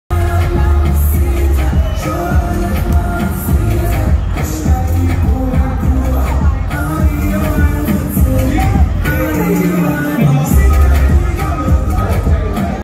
Stephane Legar Performs at Reichman University סטפן לגר מופיע באוניברסיטת רייכמן
November 2021
נובמבר 2021